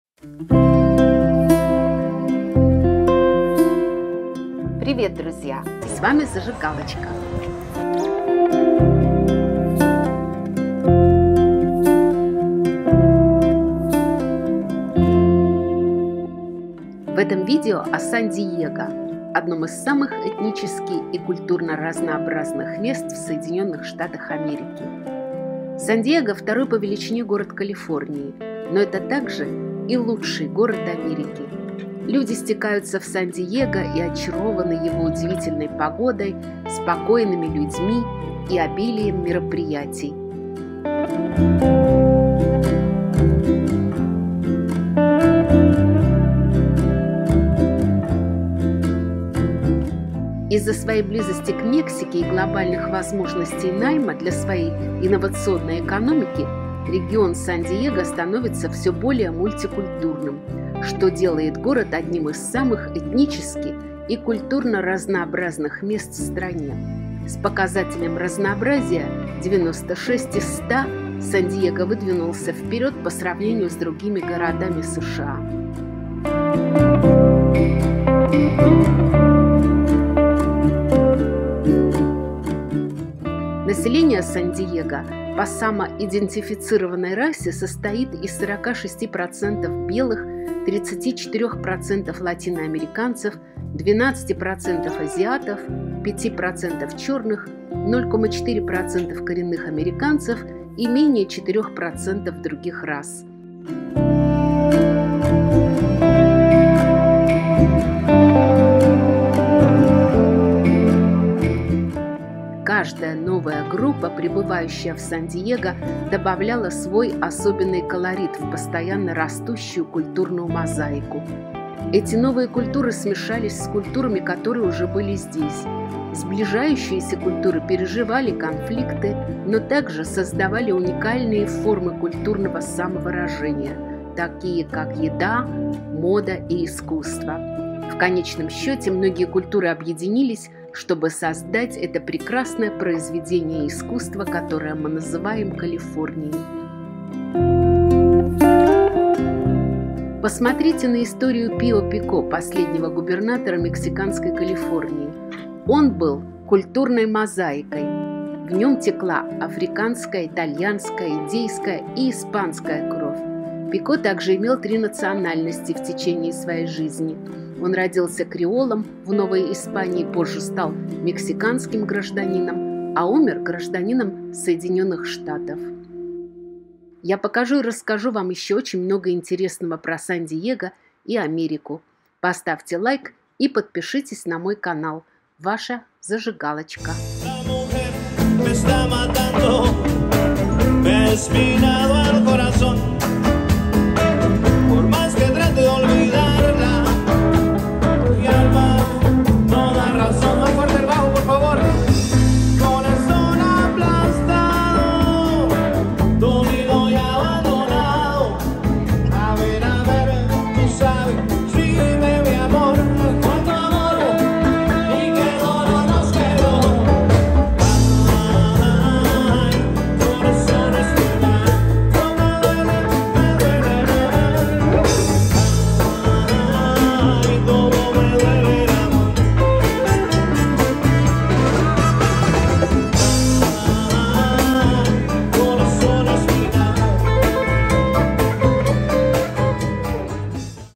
🔥Сан Диего этнически самый колоритный город США
🇺🇸 Привет, друзья! С вами 🔥Зажигалочка! В этом видео о Сан-Диего, одном из самых этнически и культурно разнообразных мест в США.

Сан-Диего второй по величине город Калифорнии, но это также и лучший город Америки. Люди стекаются в Сан-Диего и очарованы его удивительной погодой, спокойными людьми и обилием мероприятий.

Из-за своей близости к Мексике и глобальных возможностей найма для своей инновационной экономики регион Сан-Диего становится все более мультикультурным, что делает город одним из самых этнически и культурно разнообразных мест в стране. С показателем разнообразия 96 из 100, Сан-Диего выдвинулся вперед про сравнению с другими городами США.

Население Сан-Диего по самоидентифицированной расе состоит из 46% белых, 34% латиноамериканцев, 12% азиатов, 5% черных, 0,4% коренных американцев и менее 4% других рас.

Каждая новая группа, прибывающая в Сан-Диего, добавляла свой особенный колорит в постоянно растущую культурную мозаику. Эти новые культуры смешались с культурами, которые уже были здесь. Сближающиеся культуры переживали конфликты, но также создавали уникальные формы культурного самовыражения, такие как еда, мода и искусство.

В конечном счете, многие культуры объединились, чтобы создать это прекрасное произведение искусства, которое мы называем Калифорнией.

Посмотрите на историю Пио Пико, последнего губернатора мексиканской Калифорнии. Он был культурной мозаикой! В нем текла африканская, итальянская, индейская и испанская кровь. Пико также имел три национальности в течение своей жизни: он родился креолом в ​​Новой Испании, позже стал мексиканским гражданином, а умер гражданином Соединенных Штатов.

Я покажу и расскажу вам еще очень много интересного про Сан Диего и Америку.